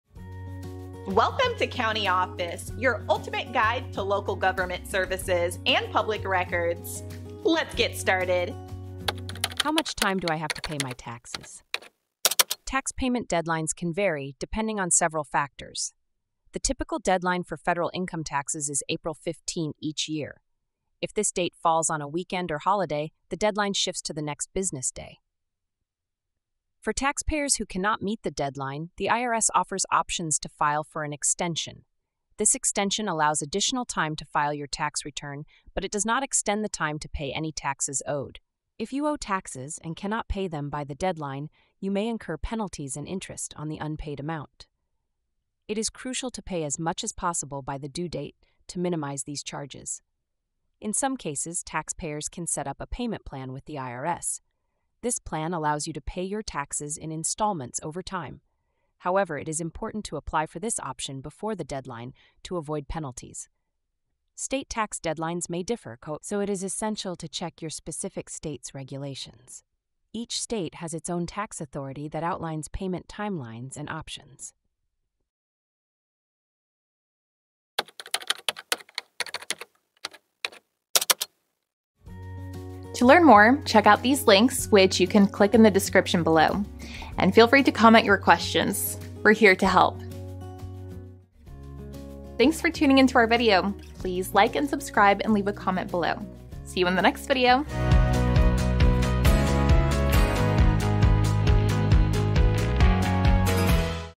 How Much Time Do I Have To Pay My Taxes? - CountyOffice.org
How Much Time Do I Have To Pay My Taxes? In this enlightening video, we delve into the intricacies of tax payment deadlines and extensions. Understanding the timelines for paying taxes is crucial, as missing deadlines can lead to penalties and interest. Learn about the typical federal income tax deadline, options for extensions, and the importance of paying as much as possible by the due date to minimize charges. Discover how taxpayers can set up payment plans with the IRS and the significance of knowing your state's specific tax regulations. Stay informed and avoid unnecessary penalties by exploring the various aspects of tax payment deadlines in this comprehensive video.